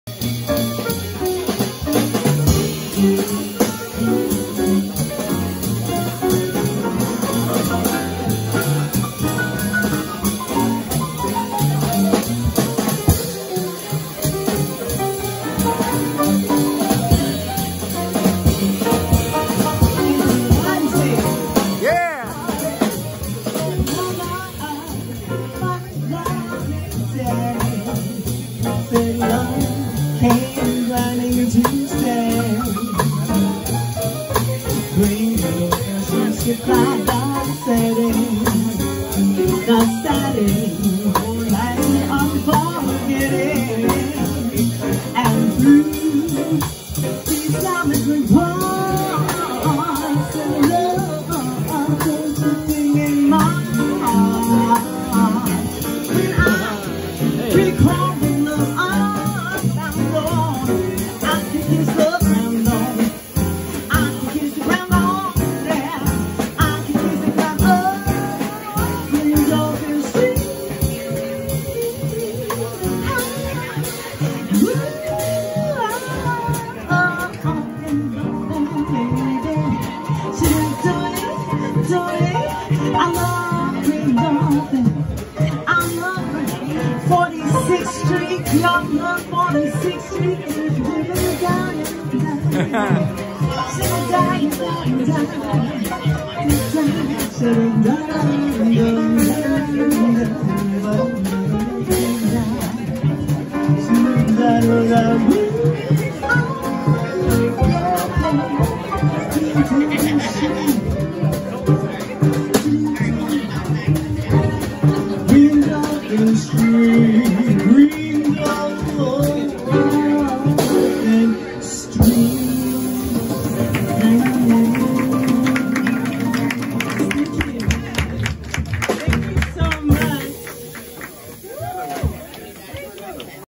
Kathryn Farmer - Le Rivage
Kathryn Farmer -  Vocal
Tyler Mitchell- Bass
Anthony Wonsey Piano
Masahiro Sakuma - Drums
Le Rivage - NYC -3/18/22